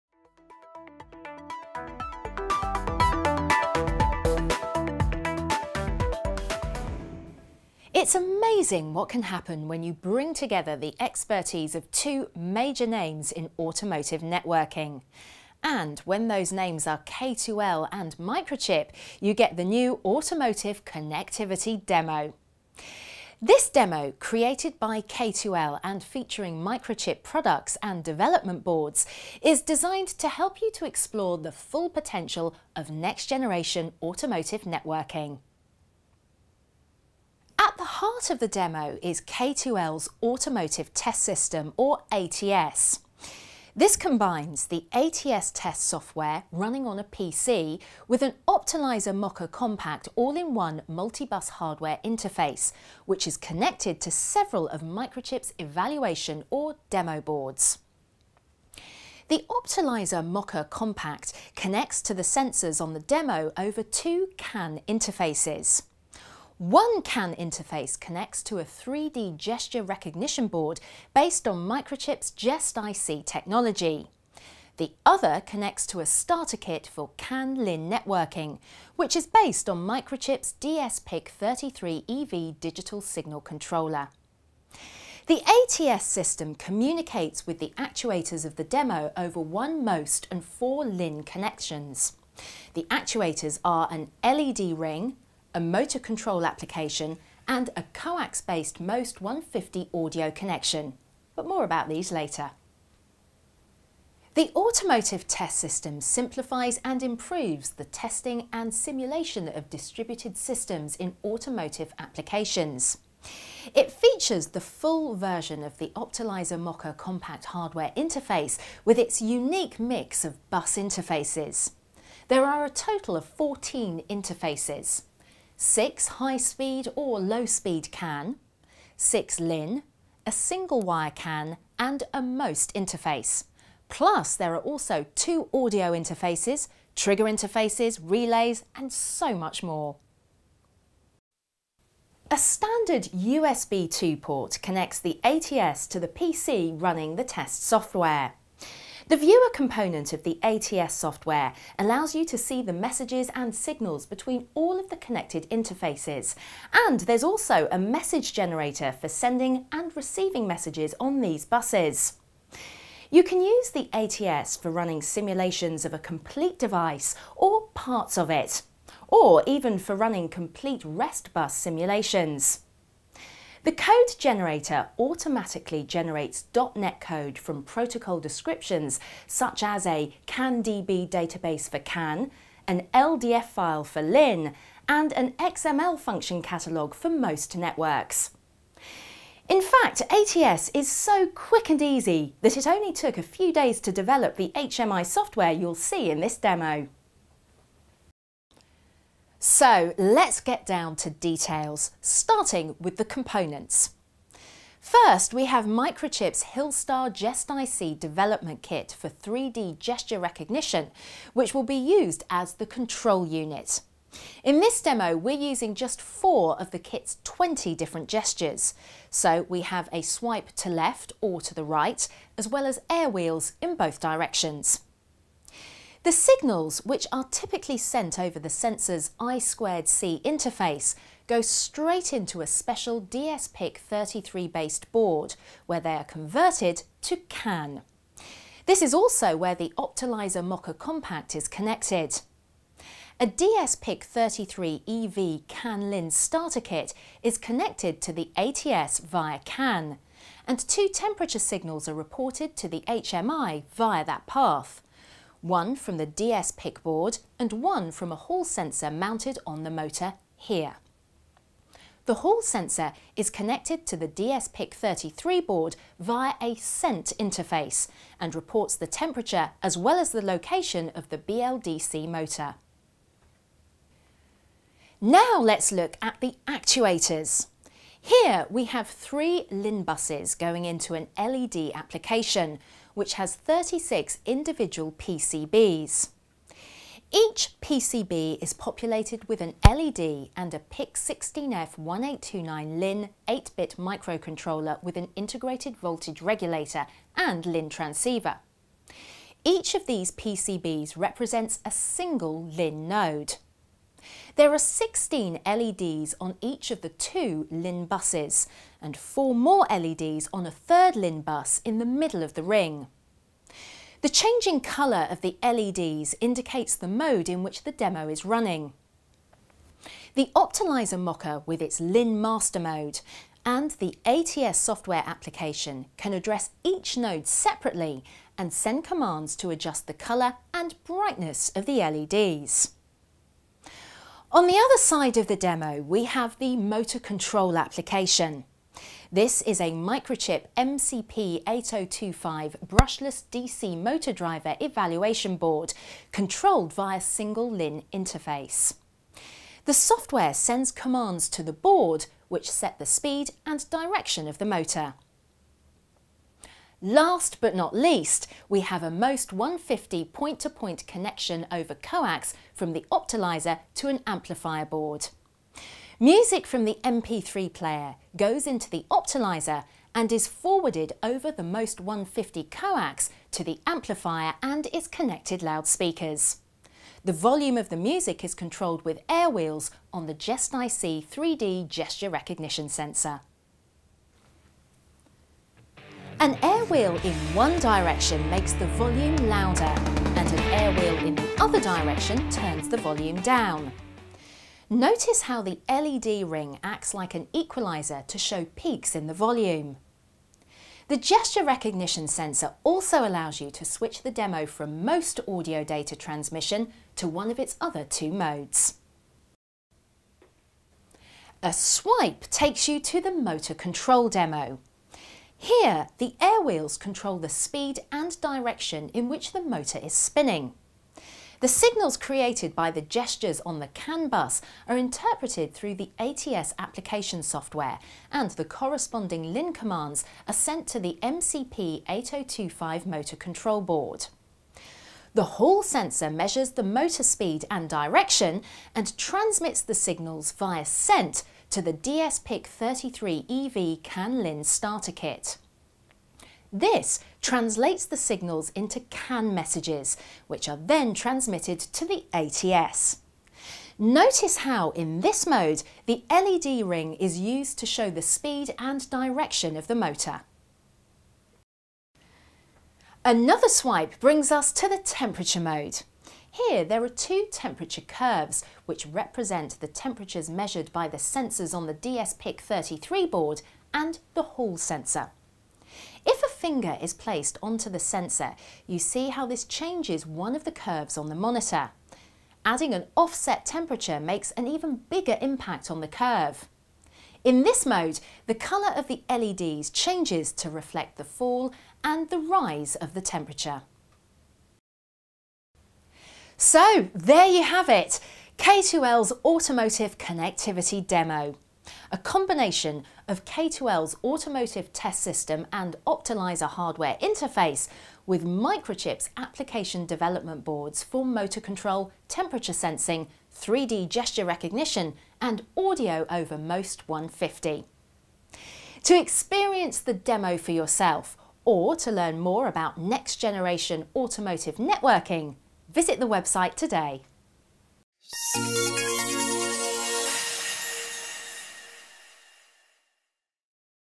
K2L Automotive Connectivity Demo
The Automotive Connectivity Demo  was developed to  a simulate an automotive distributed system based on CAN, LIN and MOST®. It shows the strength of the K2L solution Automotive Test System (ATS) working together with  Microchip´s communication and connectivity products and technologies.  The demo is operated entirely touchless, implementing Microchip’s GestIC® technology.